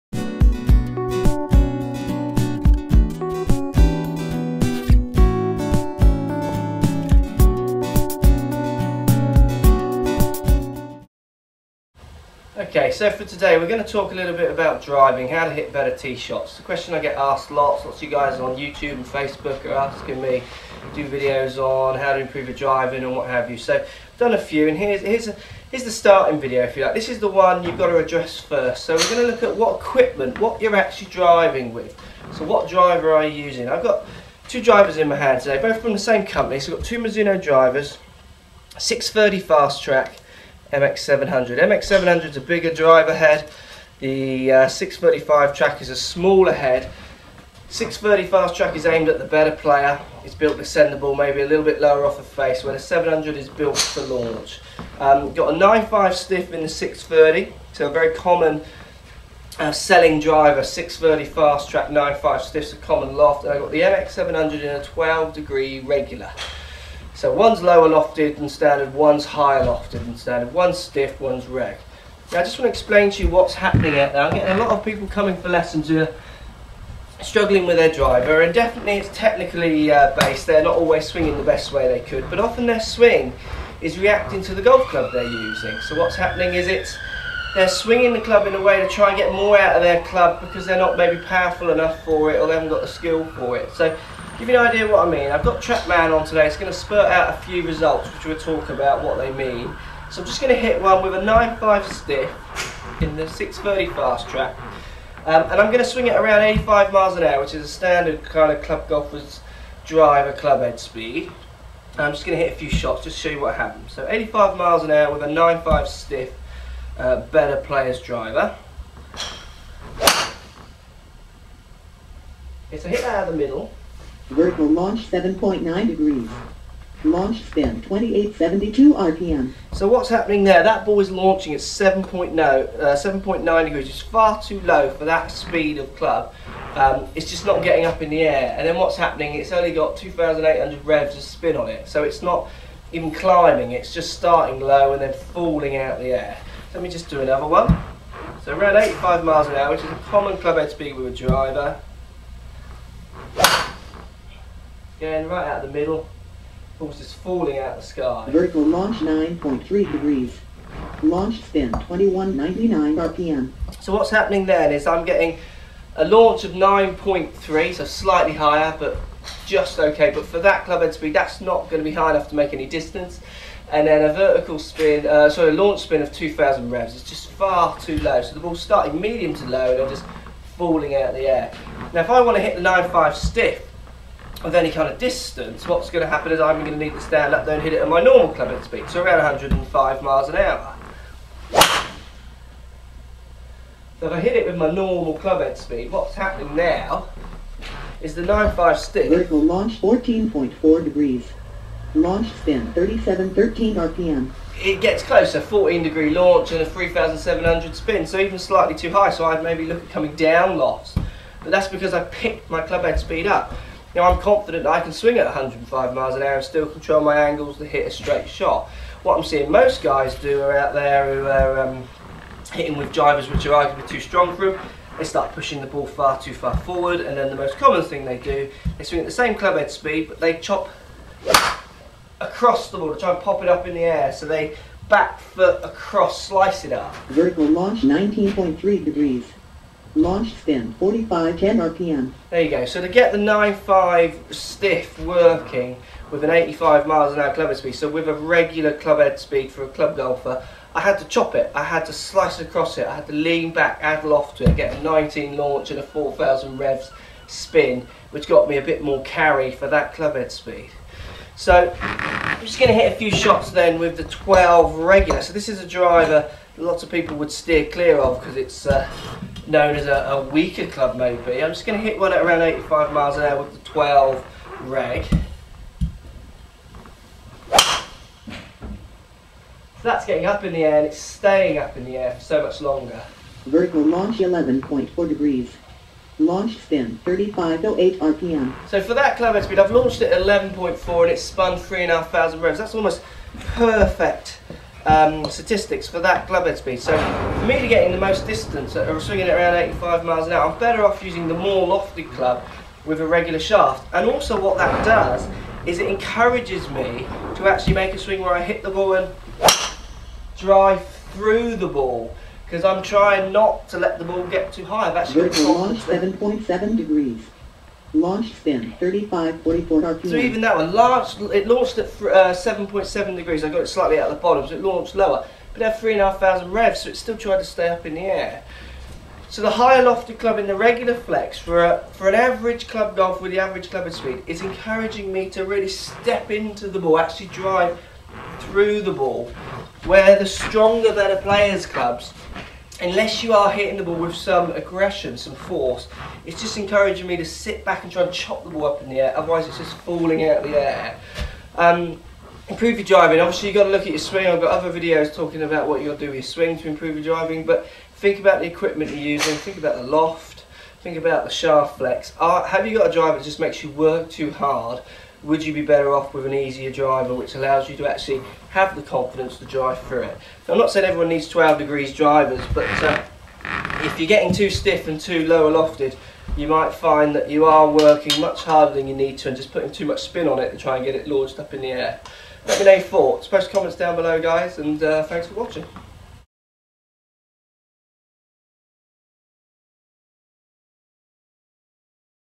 Golf Swing Tips | Golf lesson | Tee Shot Lesson
Golf swing tip from the Exeter golf professional Mark Crossfield, watch the free video golf lesson and improve your golf driving and tee shots. Choose the correct club to make your best golf swing.